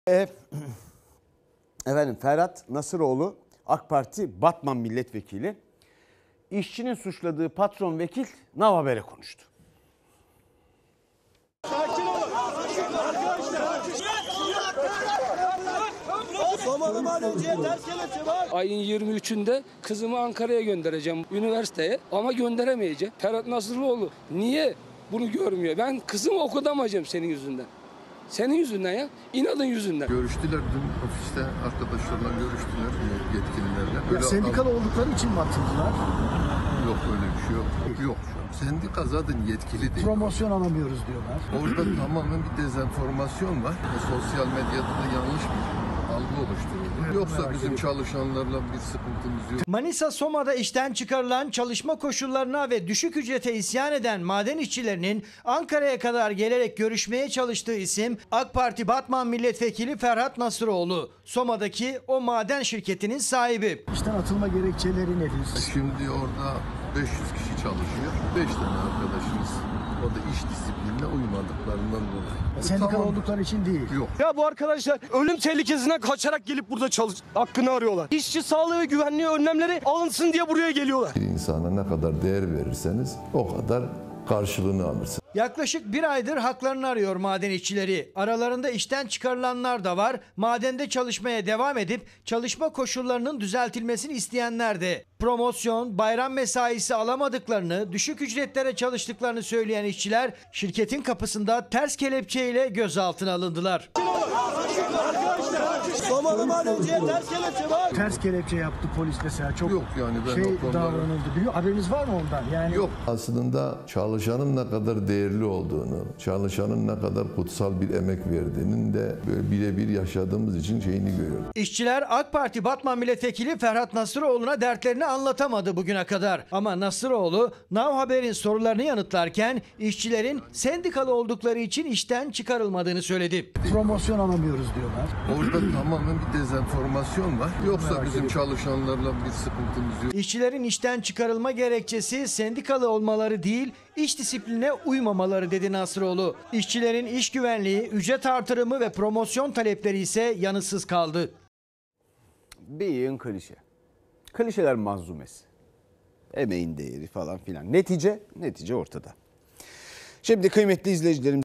Batmanlı Vekil, işçi eylemleri için ilk kez konuştu
“İş disiplinine uymadılar”
Manisa’nın Soma İlçesinde Nasıroğlu ailesine ait Fernas madencilik şirketinde işten çıkarılan 5 işçi için eylemler sürerken, eleştirilerin hedefinde olan AK Parti Batman Milletvekili Ferhat Nasıroğlu da konuyla ilgili ilk kez konuştu.
BATMANLI VEKİL, AİLE ŞİRKETİYLE İLGİLİ ELEŞTİRİLERE İLK KEZ YANIT VERDİ
Nasıroğlu ailesine ait Fernas madencilikte 5 işçinin işten çıkarılması, günlerce eleştiri konusu oldu. Ulusal televizyon kanalları, işine son verilen işçilerin Bağımsız Maden İşçileri (Bağımsız Maden İş) Sendikasına üye olduğunu ileri sürdü. Eleştirilerin hedefinde olan AK Parti Batman Milletvekili Ferhat Nasıroğlu, konuyla ilgili ilk kez konuştu.
“ÇALIŞANLARLA SIKINTIMIZ YOK”
Ankara’da NOW TV muhabirinin sorularına yanıt veren Nasıroğlu, Soma’da bulunan Fernas madencilik şirketinde 500 çalışanları olduğunu ve hiçbir işçi ile sorunlarının olmadığını belirtti. 5 işçinin işten çıkarılma gerekçesini açıklayan Nasıroğlu “Orada 500 kişi çalışıyor. 5 tane arkadaşımız. Orada iş disiplinine uymadıklarından dolayı işten çıkarıldı. Orada tamamen dezenformasyon var. Yoksa bizim çalışanlarla bir sıkıntımız yok. İşçiler sendikalı oldukları için değil, iş disiplinine uymadıkları için işten çıkarıldı” dedi. (Ramazan Demir)